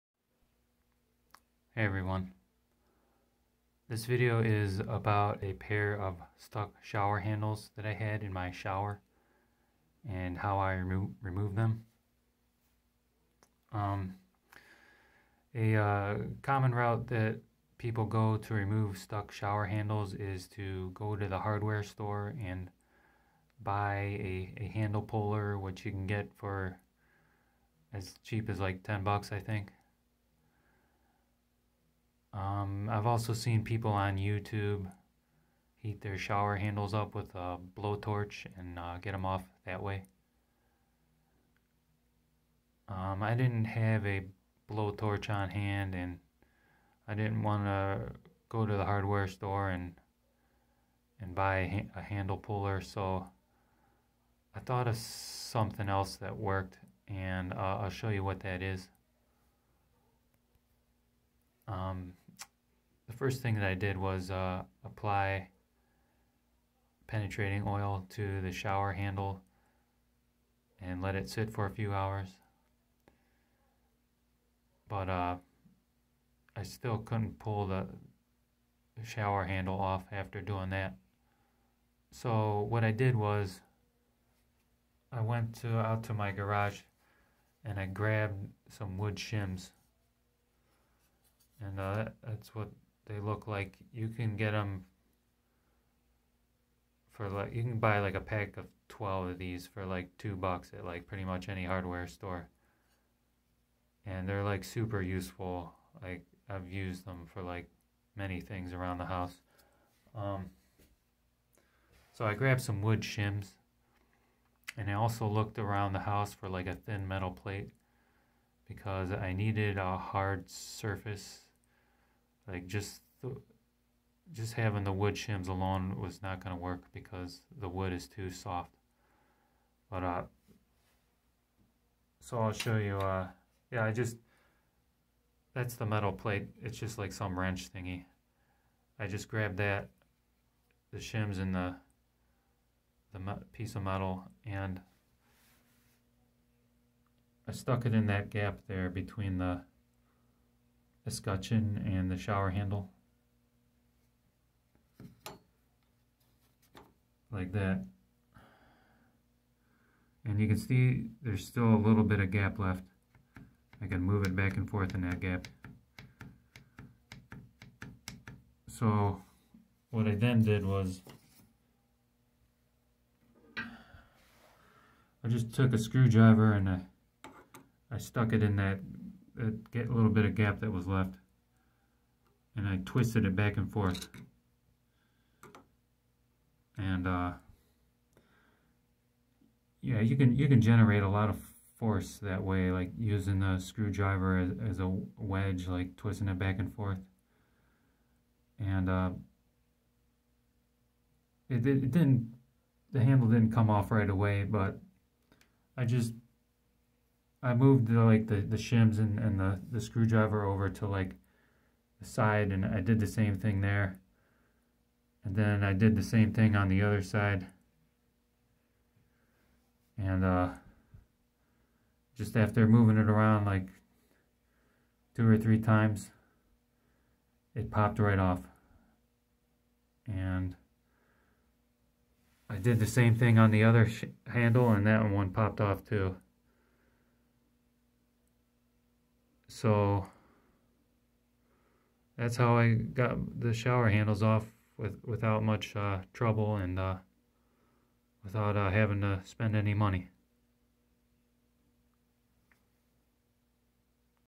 How I removed a stuck shower handle without using a handle puller or blowtorch
IMPORTANT: Make sure to remove the shower handle screws located underneath the handle end caps before taking the handles off.

This video shows how to remove a stuck shower handle using common household items.